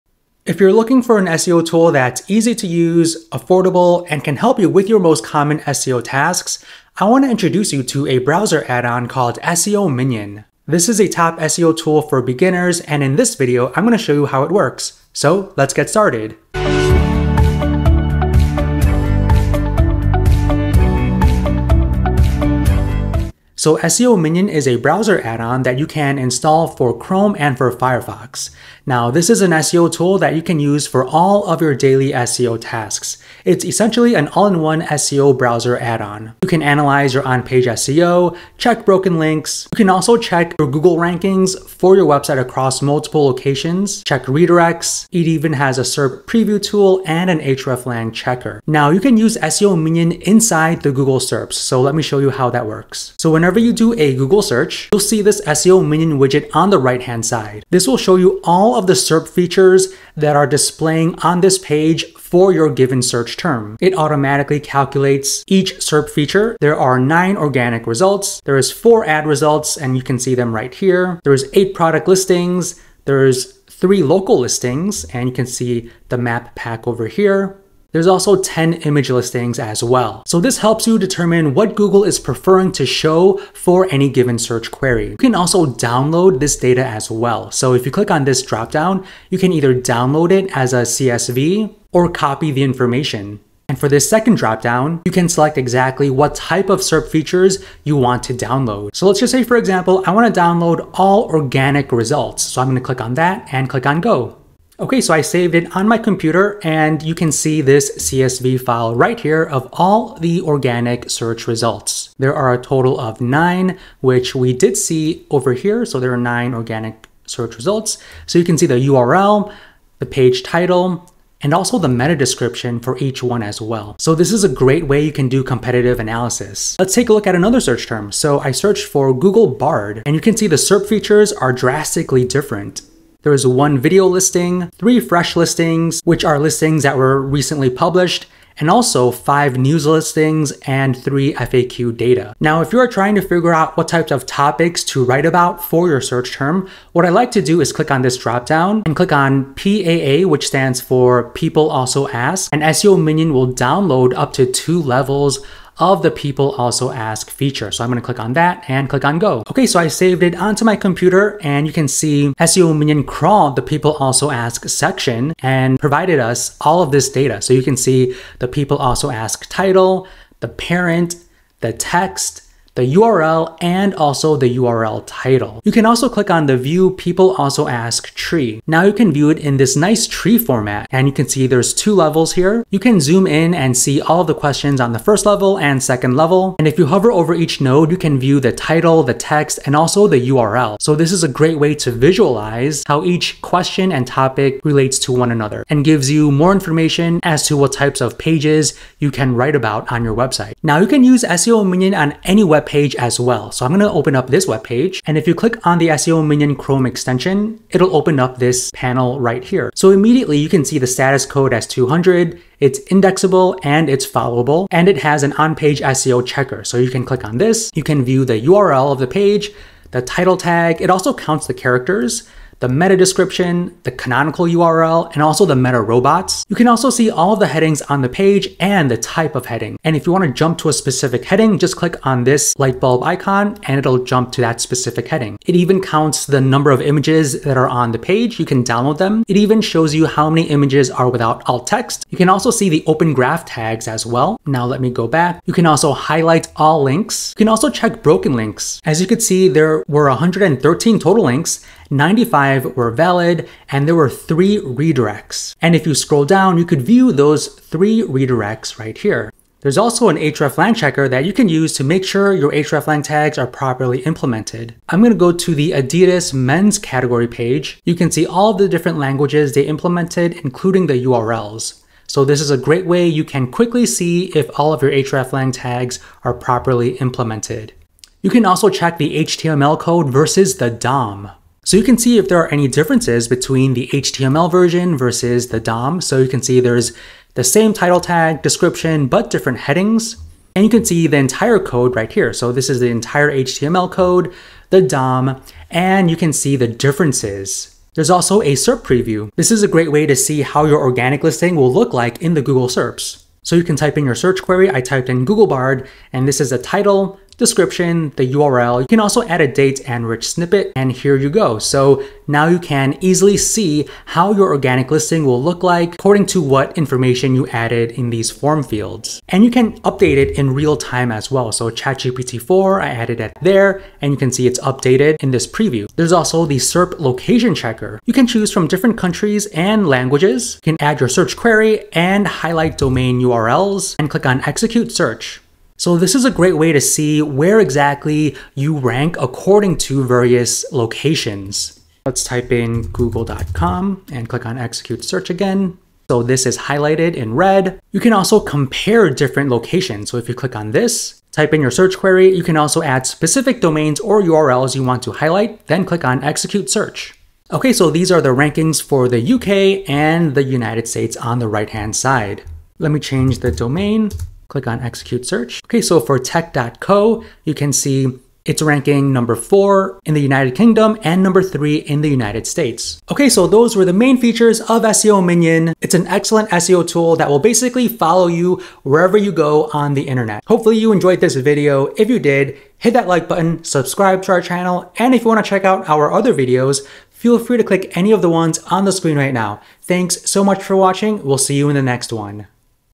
Top SEO Tool in 2023 - SEO Minion
SEO Minion is a top SEO tool for all your daily SEO tasks. It's a browser add-on that you can use across websites with all the popular SEO features at your fingertips.